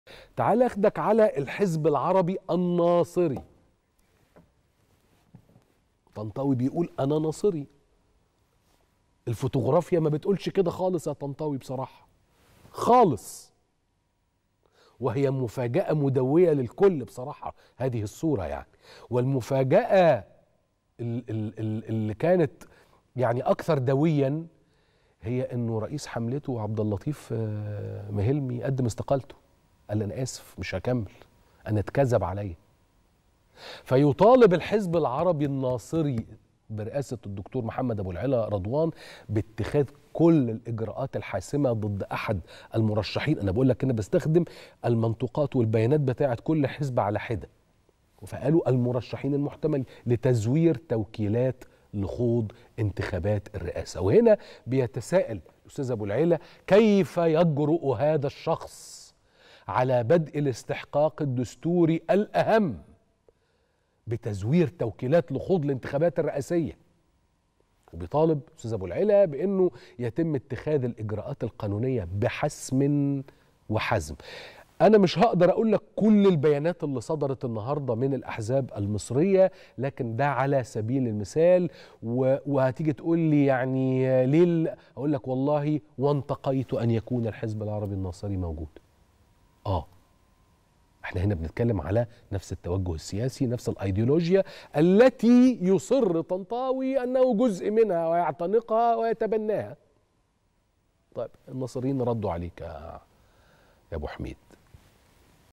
الحزب العربي الناصري يطالب باتخاذ إجراءات حاسمة ضد مرشح محتمل زور توكيلات لخوض انتخابات الرئاسة
التاسعة | الحزب العربي الناصري يطالب باتخاذ إجراءات حاسمة ضد مرشح محتمل زور توكيلات لخوض انتخابات الرئاسة